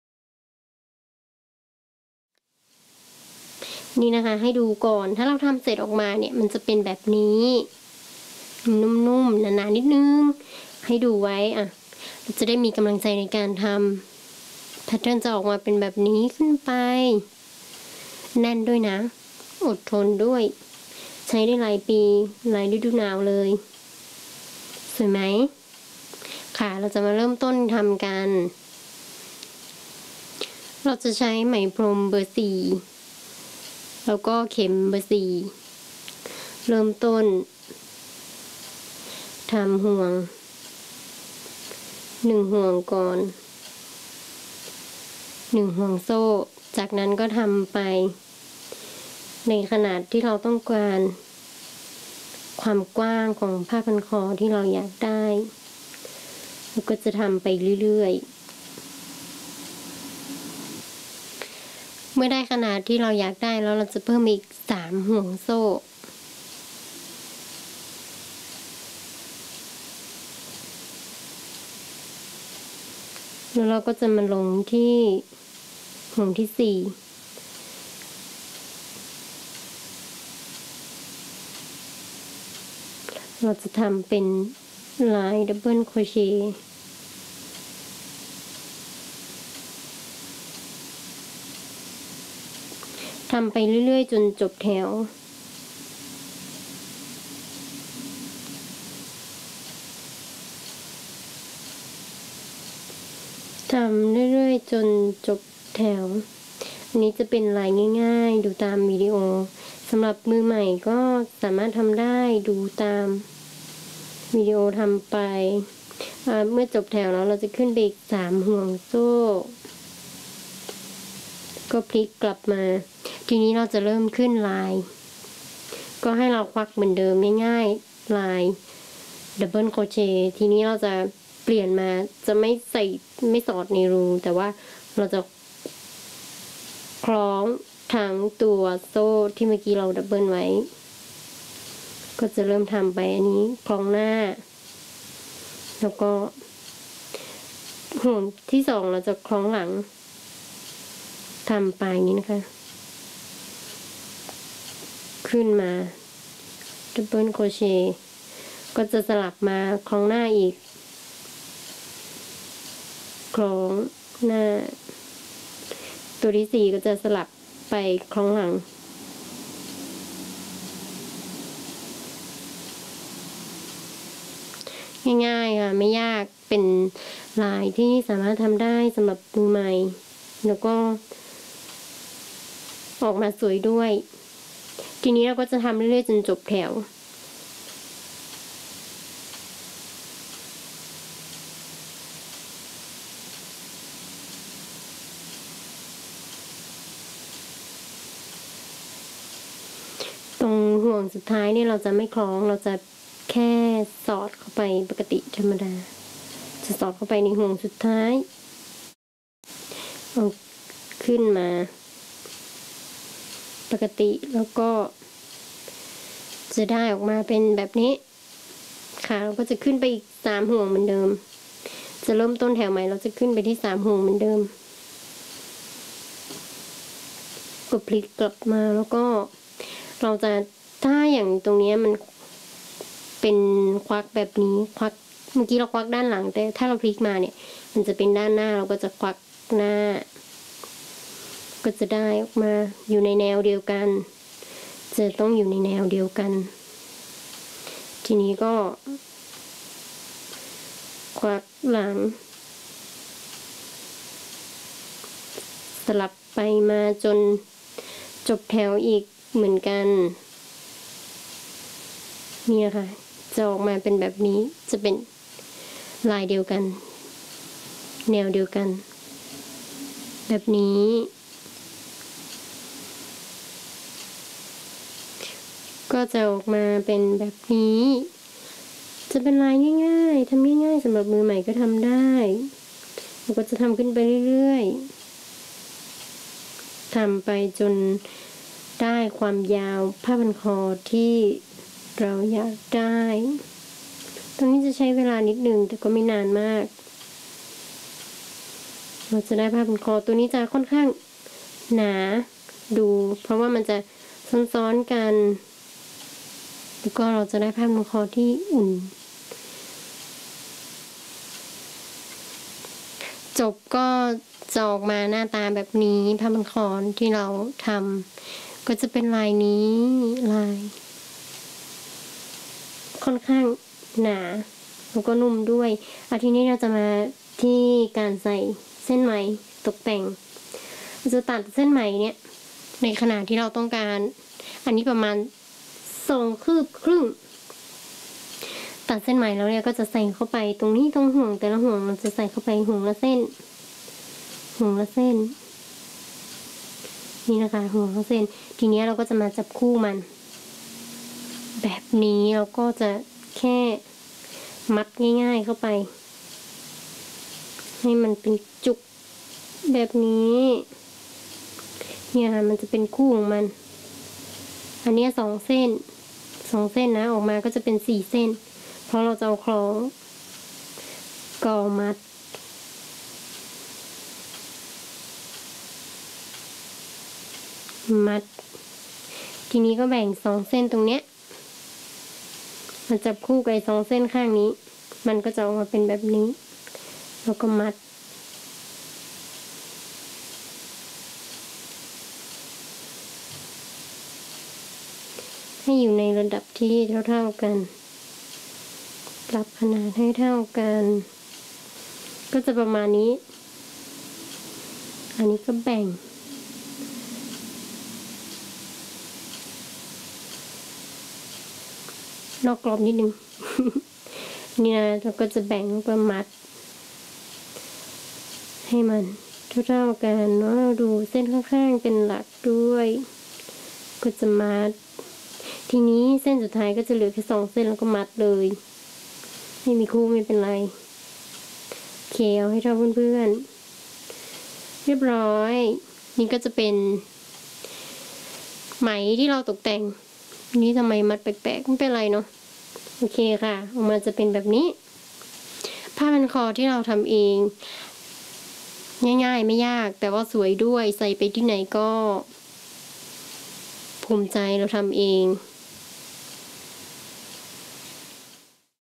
Easy Scarf Crochet (Eng sub) ผ้าพันคอโครเชต์ เข้าใจง่ายใน7นาที | Maisie Crochet
ผ้าพันคอโครเชต์ เข้าใจง่ายใน7นาที | Maisie Crochet
สอนถักผ้าพันคอโครเชต์ ลายถักพ2ค หรือลายถักF ทั้งผืน
วัสดุที่ใช้
1. ไหมพรม เบอร์4  จำนวน250กรัม
2. เข็มโครเชต์ เบอร์4.25

*สามารถอวดผลงานได้ที่ เพจ